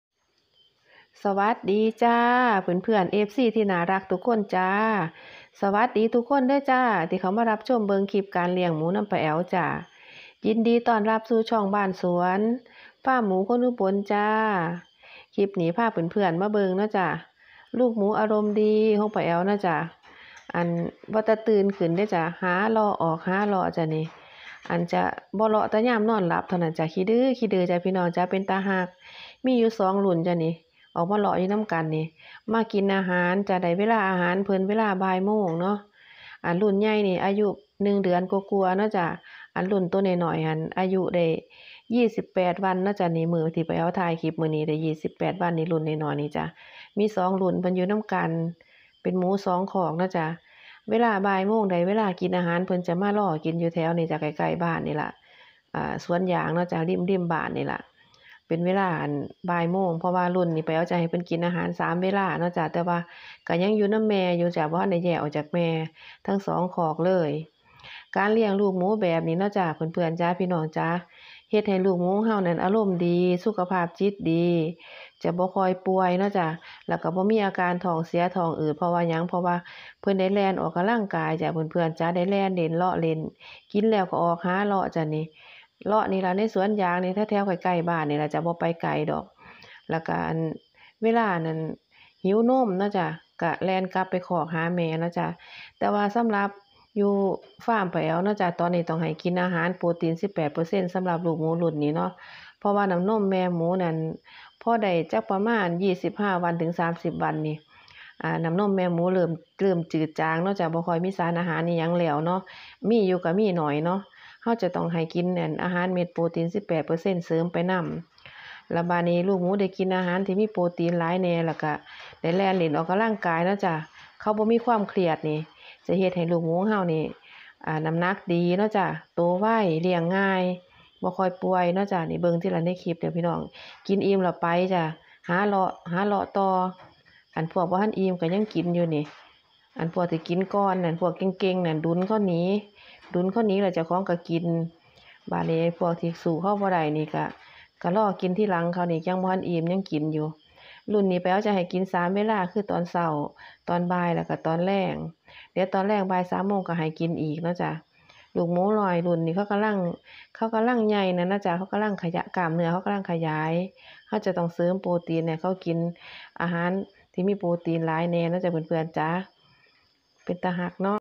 ลูกหมูอารมณ์ดี,เลี้ยงง่าย,สุขภาพดีโตไว#ป้าแอ๋วคนเลี้ยงหมู
ลูกหมูสายพันธุ์ป่าลูกผสม เลี้ยงง่าย,ลูกหมูอารมณ์ดี สุขภาพดีโตไว